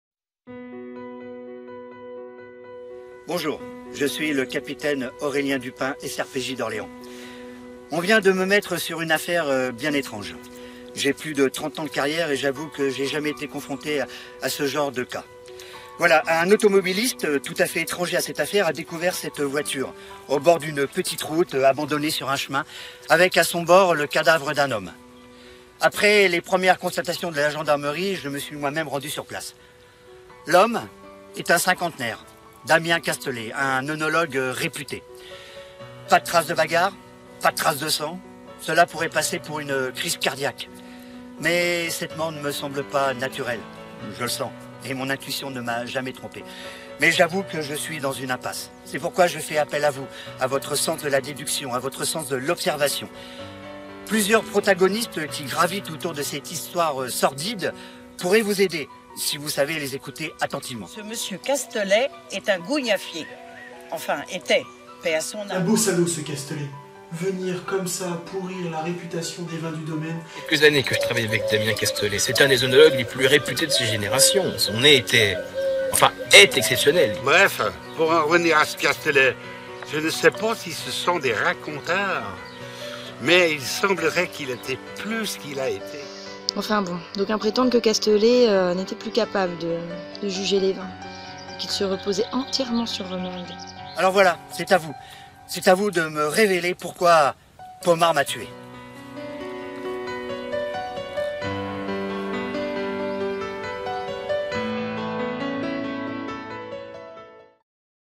POMMARD M'A TUER
"Pommard m'a tuer" ! Un crime de sang secoue le petit monde du vin. Et pour cause, un de ses plus célèbres critiques a été retrouvé mort dans sa voiture. Venez mener cette enquête policière et oenologique, entre collègues, au cours d'un séminaire d'entreprise.